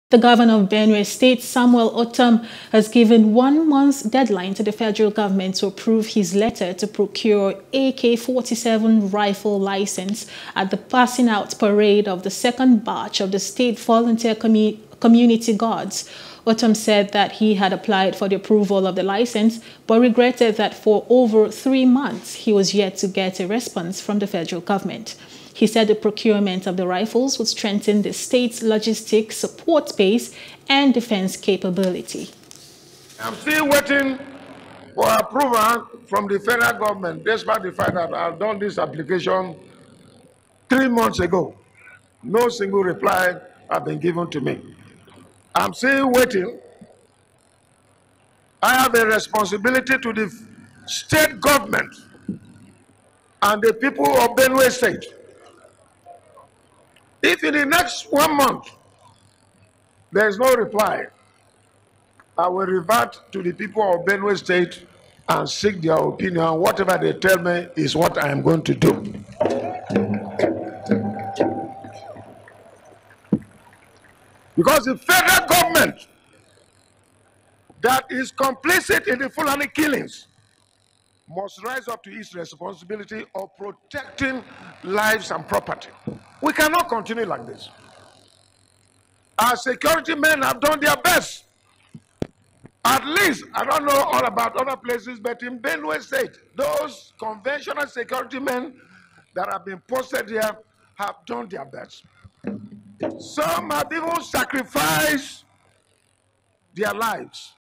Ortom Gives Fed Govt One Month Dealdine To Approve AK-47 Licence | NEWS
The Governor of Benue state, Samuel Ortom has given one-month deadline to the federal government to approve his letter to procure AK-47 riffle licence.